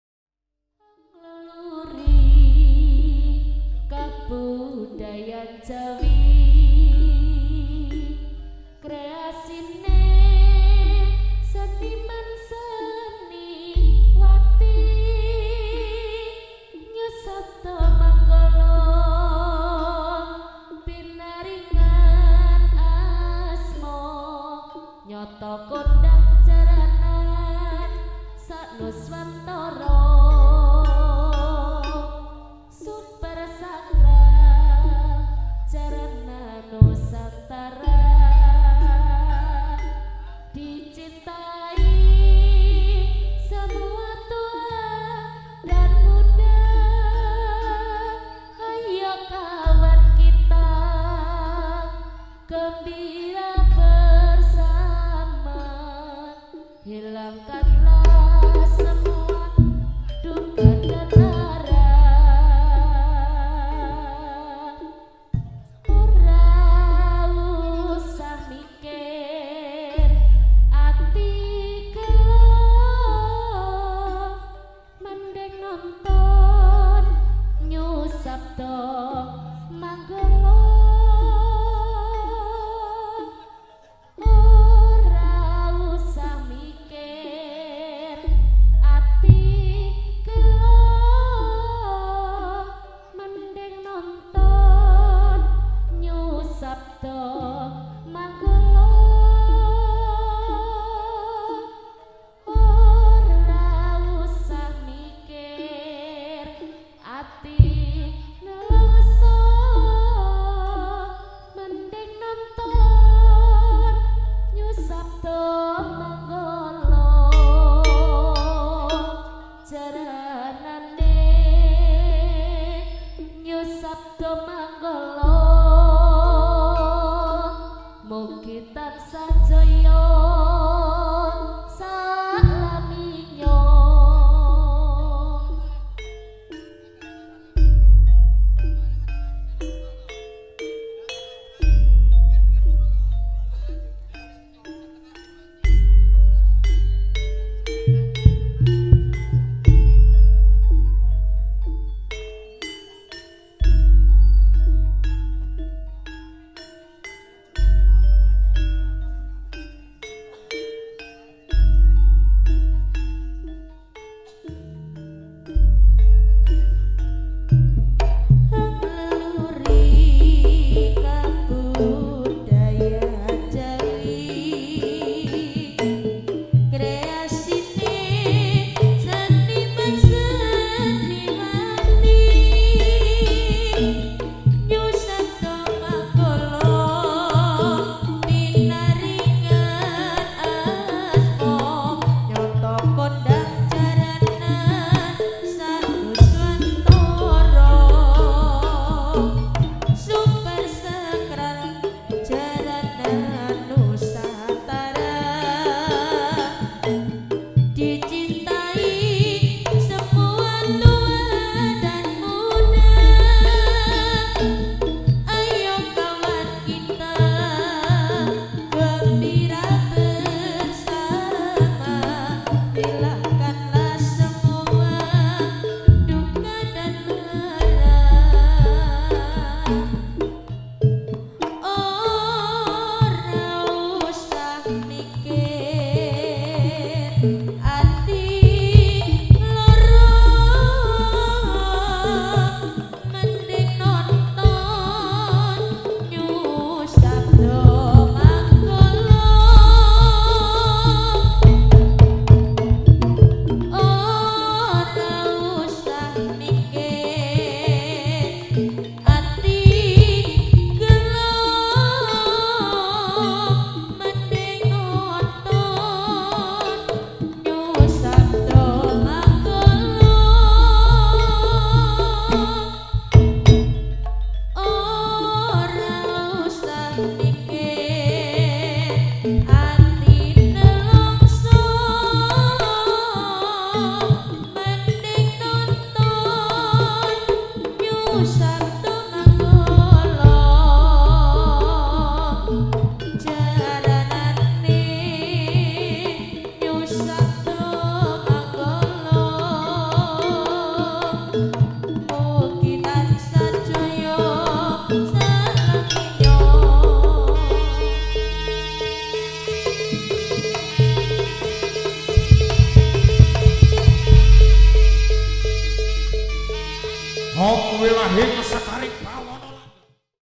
Suguh Sesaji Super Sakral - NEW SABDO MANGGOLO Live JETIS WARUJAYENG 2019
Suguh Sesaji Sakral Pambuko - NEW SABDO MANGGOLO Live JETIS WARUJAYENG 2019. Didukung Oleh JORDAN AUDIO PRO.

Salam Budaya - Pesona Jaranan Indonesia !!!